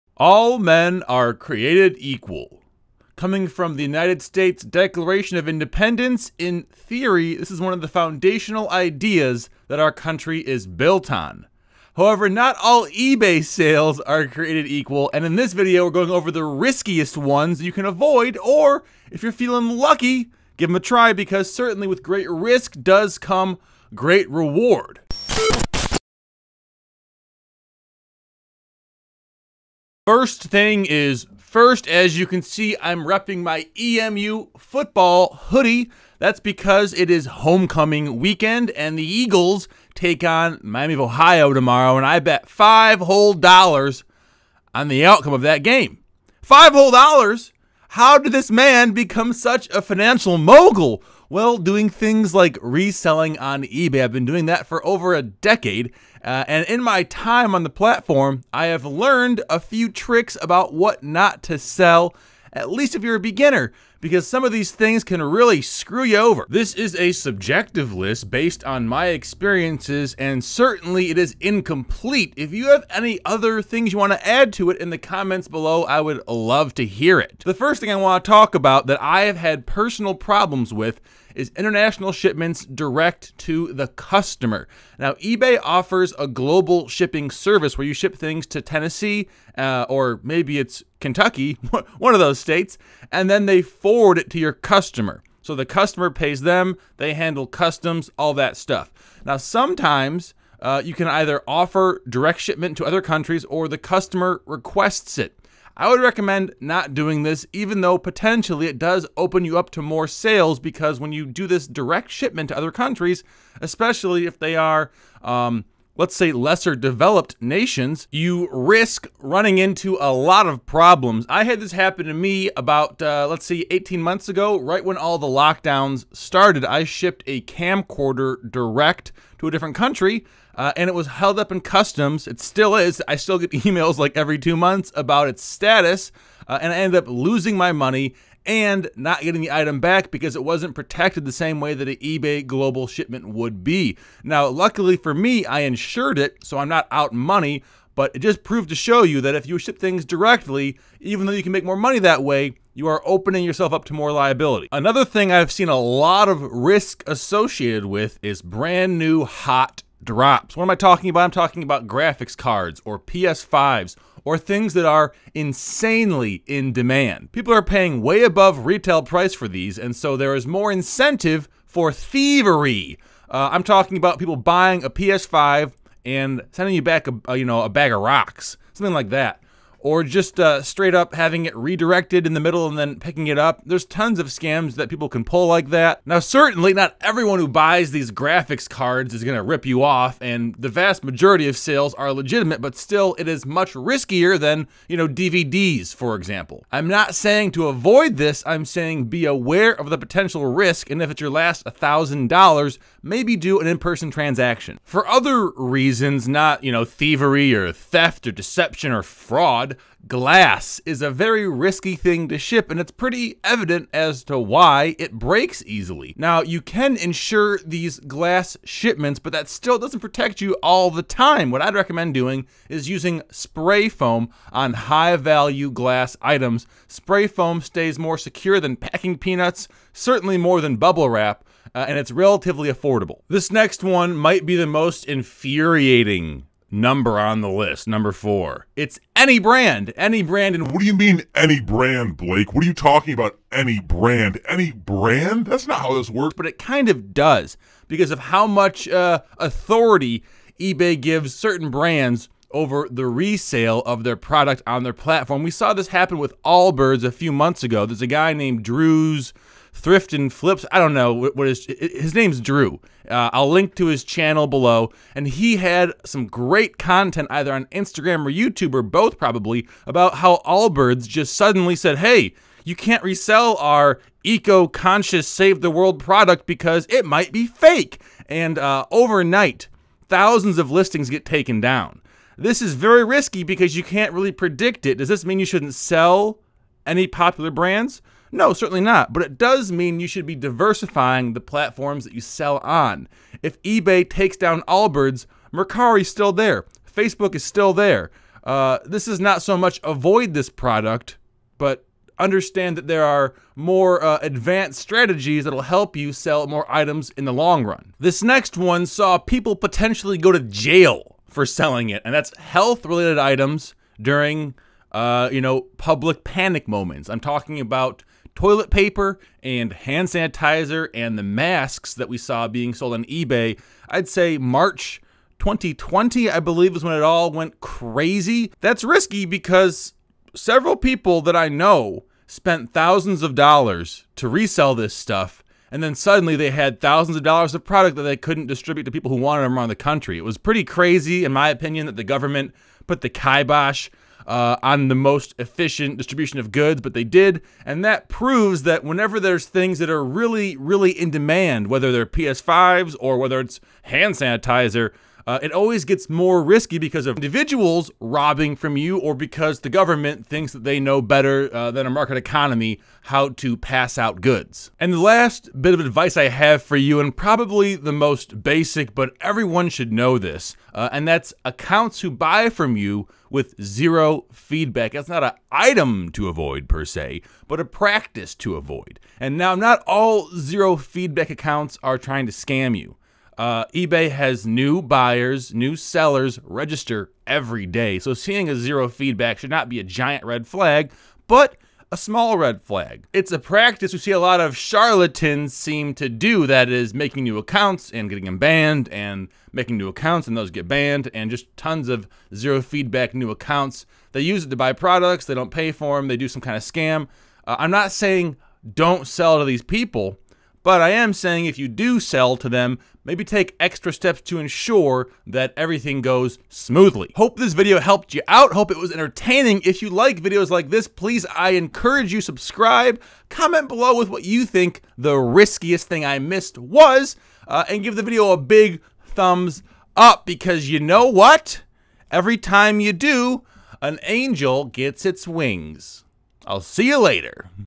The RISKIEST Things to Sell on eBay [And How to Protect Yourself]
This stuff is high-risk, high-reward, so make sure you know what you're getting into if you don't have money to lose.

WBKUltra.com (coming soon)